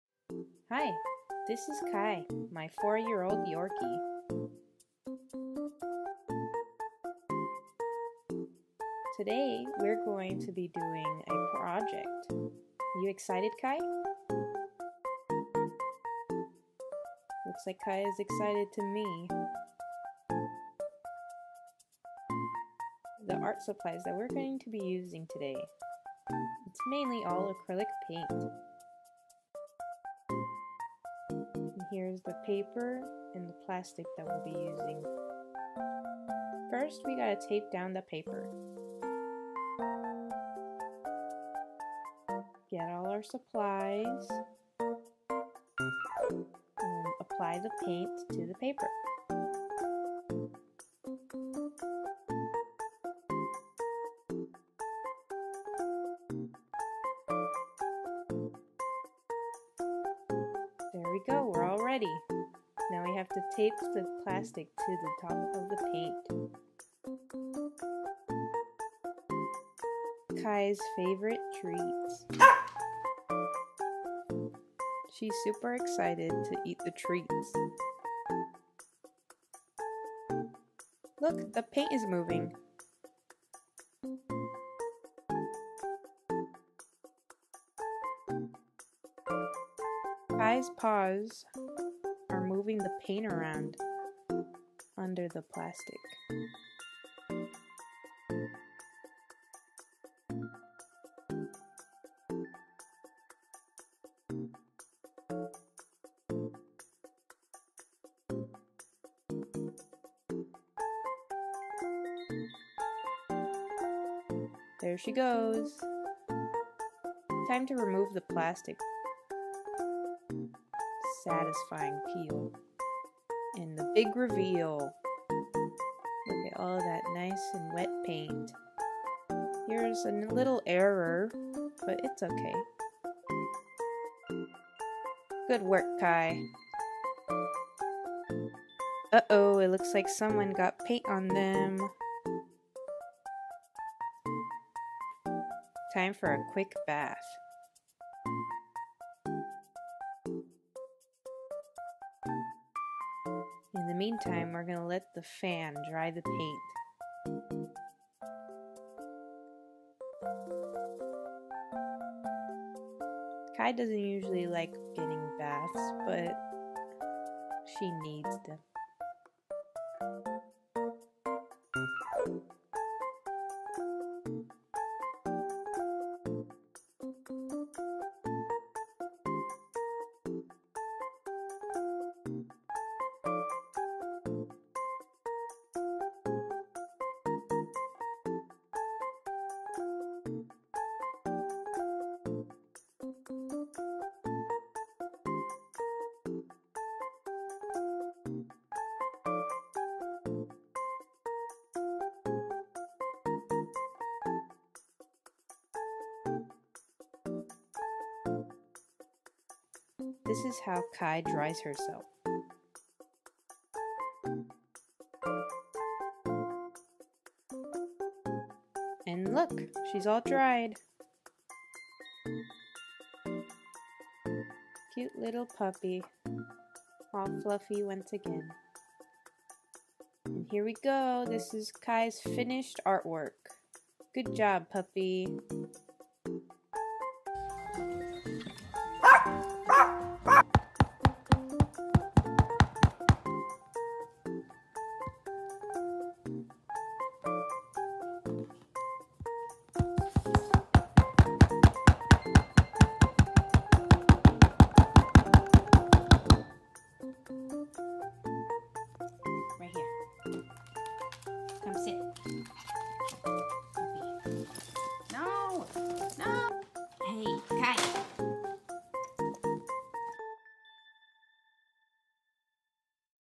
Puppy's Painting Palooza - Episode One
Welcome to the first episode of Puppy's Painting Palooza! Meet my 4-year-old Yorkie named Kai, and watch as she makes amazing works of art!